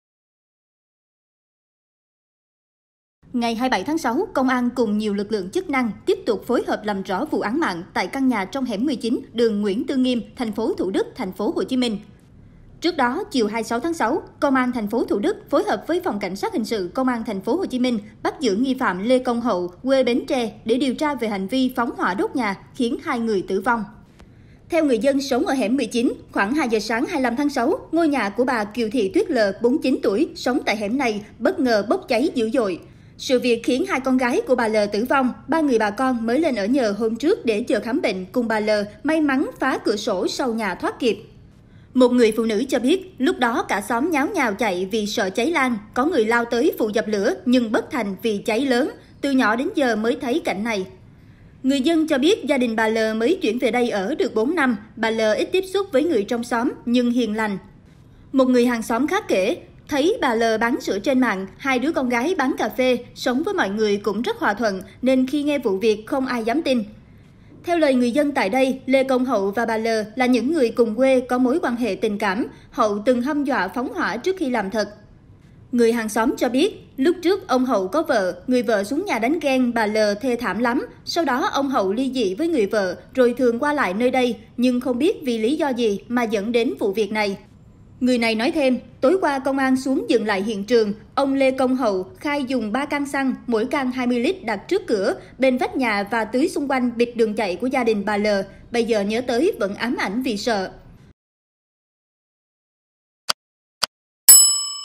Lời kể hàng xóm chứng kiến vụ phóng hỏa làm chết 2 chị em ruột | Báo Người Lao Động
Đau xót và ám ảnh là tâm trạng của người dân sống tại hẻm 19, đường Nguyễn Tư Nghiêm khi nhắc về vụ phóng hỏa làm 2 chị em ruột thiệt mạng.

Link bài viết trên Báo Người Lao Ðộng - MỜI ĐỌC

* Thực hiện: Ngọc Trinh - Ngọc Lý